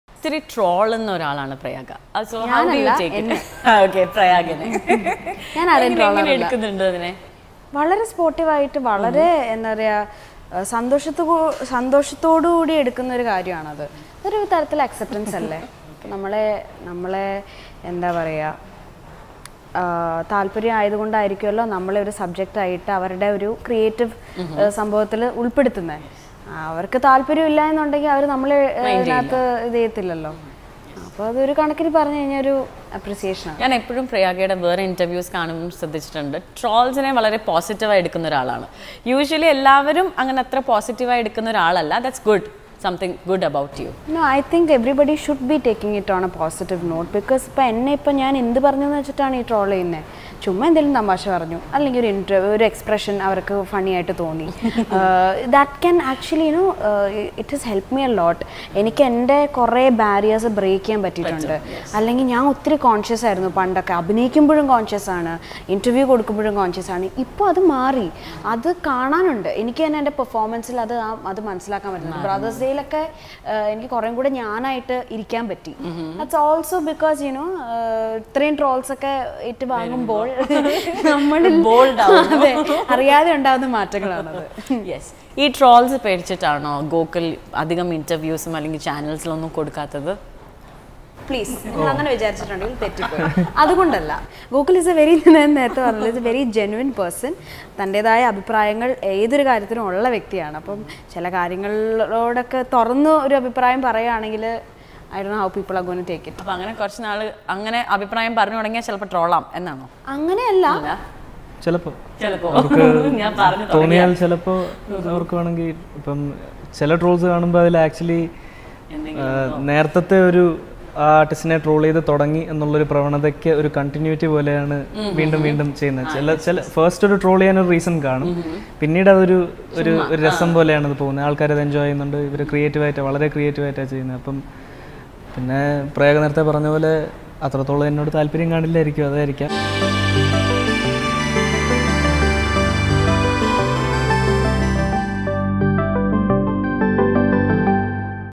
What Gokul Suresh and Prayaga Martin have to say about trolls | Kaumudy
Gokul Suresh is an Indian film actor who appears in Malayalam films. He is the son of actor-politician Suresh Gopi. He graduated with a bachelor's degree in Business Administration from Christ University and did internship at Le Méridien, Kochi before entering into cinema.

Ulta is an upcoming Indian Malayalam-language comedy drama film written and directed by Suresh Poduval and produced by Dr. Subash Cipy under the production banner Cipy Creative Works Pvt. Ltd